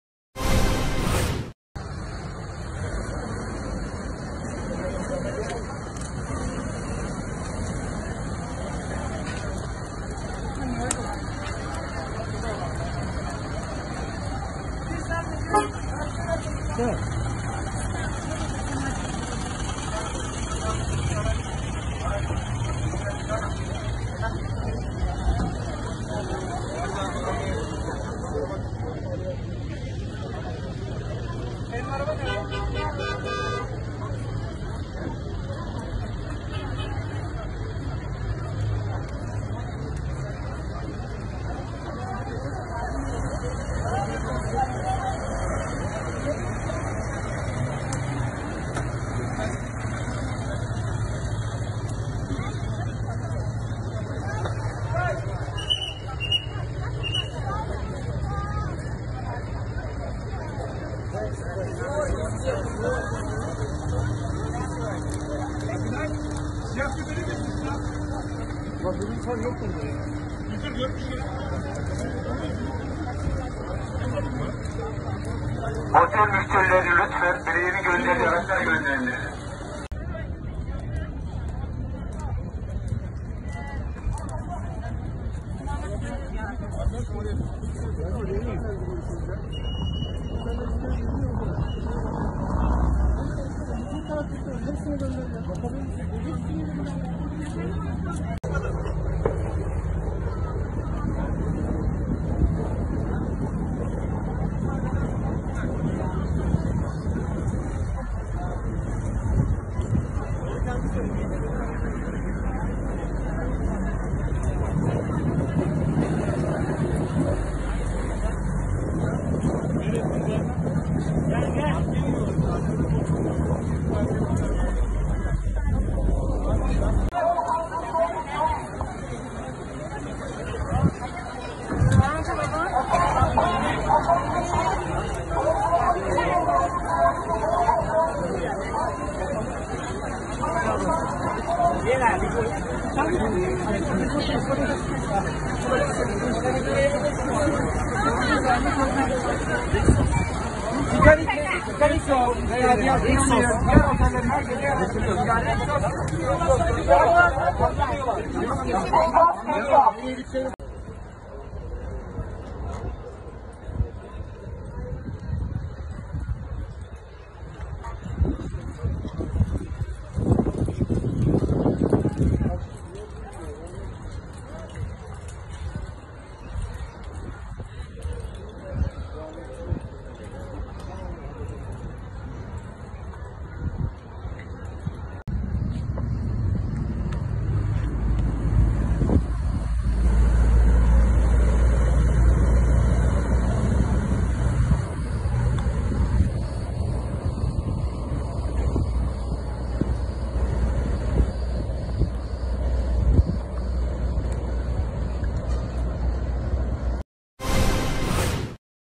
В Бодруме горят элитные отели
В Бодруме горят элитные отели. Дорога расплавилась от огня. Все яхты города плывут забирать людей по морю